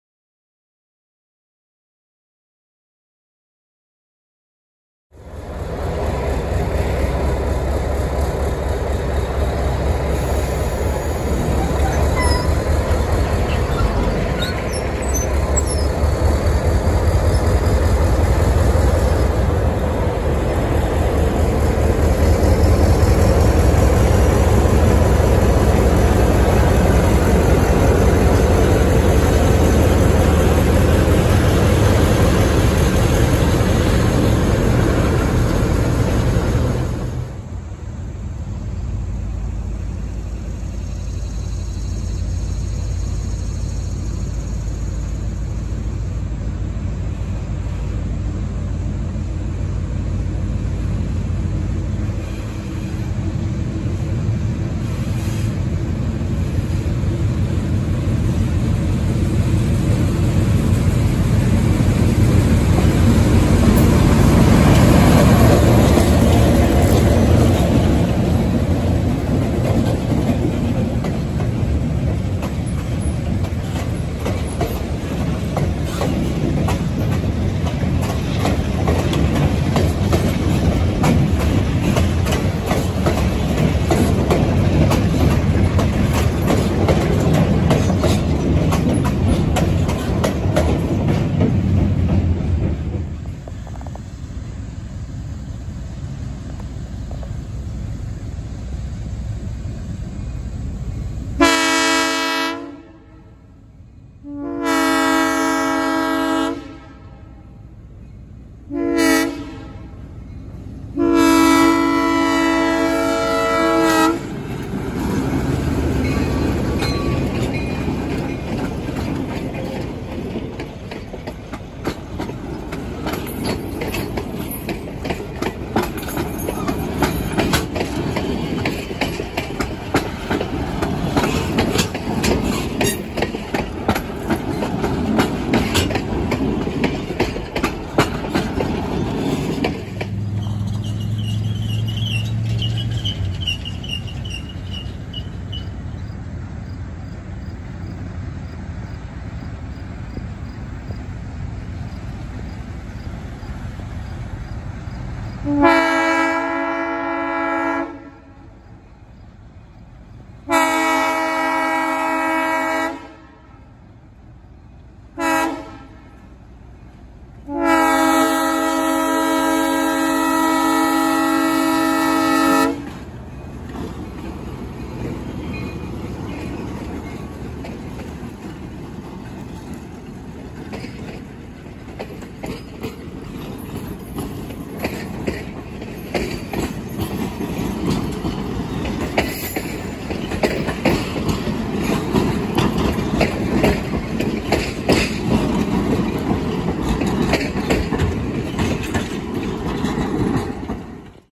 Limas to Metamora | Whitewater Valley Railroad 709 and 25
This was one of 709’s first public excursions. She took the cowboy train to Metamora and back on this day. Although she still looks a bit rough, she’s been running great so far! This was recorded on August 19, 2023.